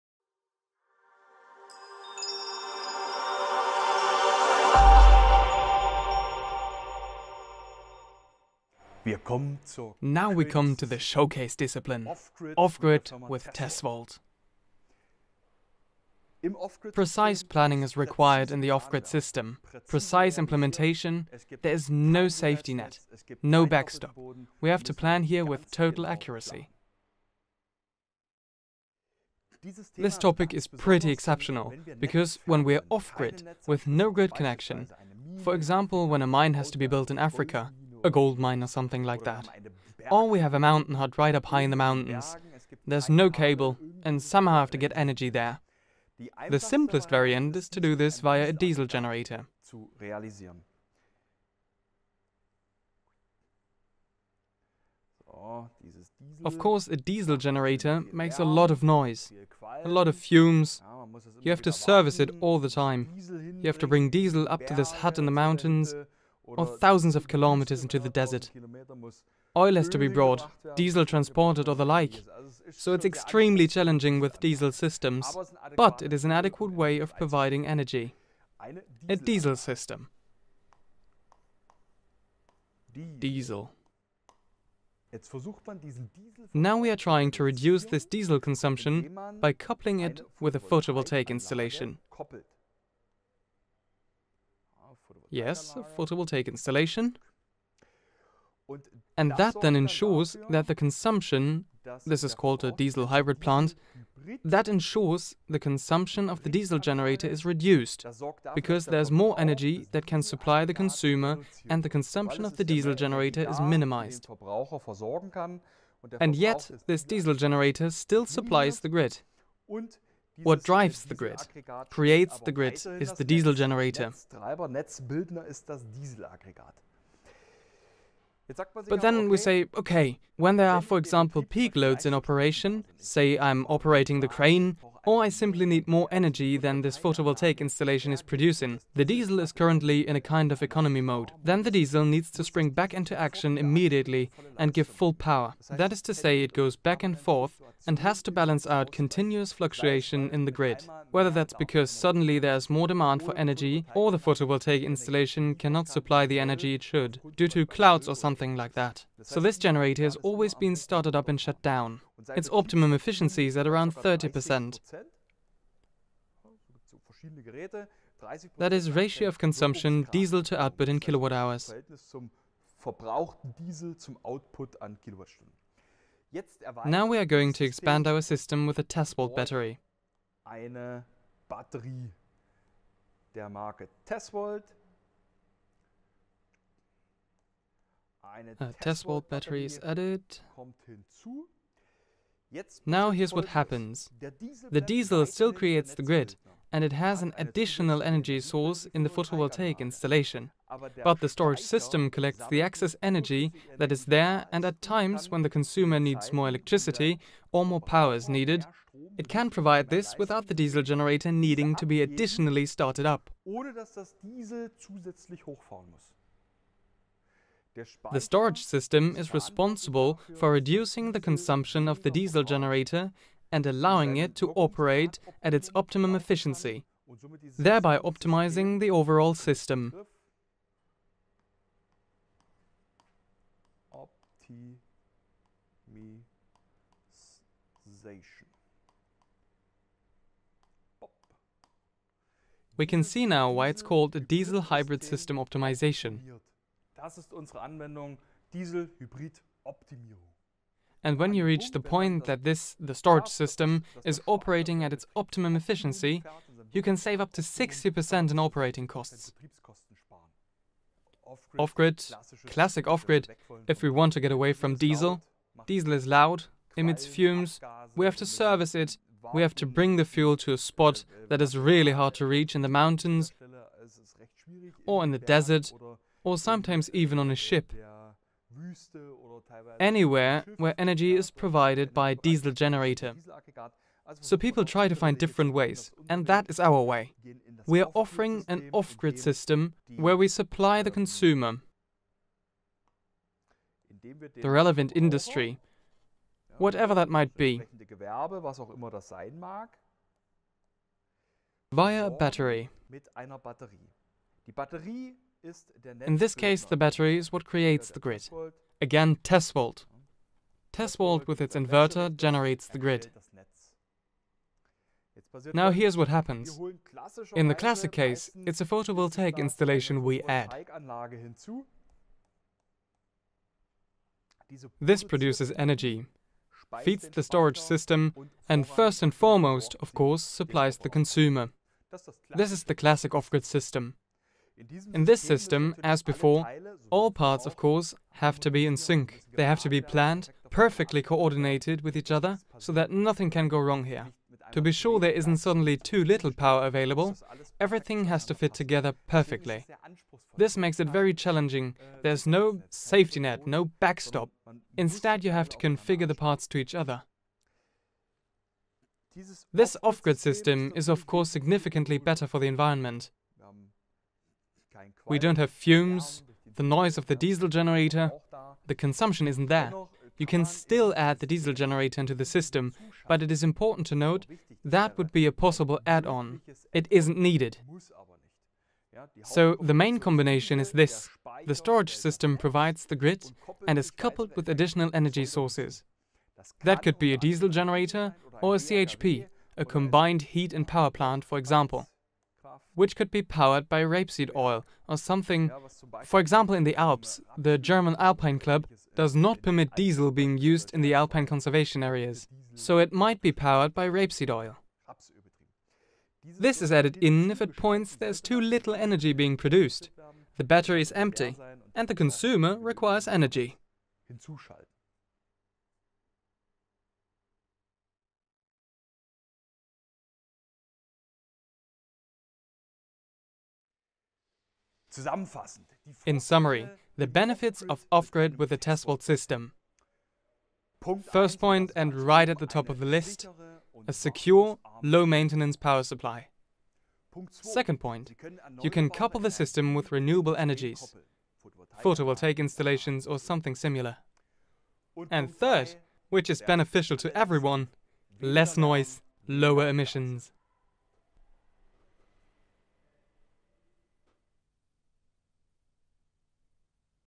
Off Grid (ENG)
If there is no grid in an area, but energy is needed there, the question arises: How can I get energy there? This is where the off-grid system comes into play. Mathias Zdzieblowski explains what this is in this video.

The classic off-grid system consists of a consumer that needs to be supplied with energy, a photovoltaic system and a Tesvolt battery storage system. Here it is particularly important that all areas are perfectly coordinated with each other.

The advantages of an off-grid system with our battery storage systems are that there is less noise and exhaust fumes and the system is safe and requires little maintenance.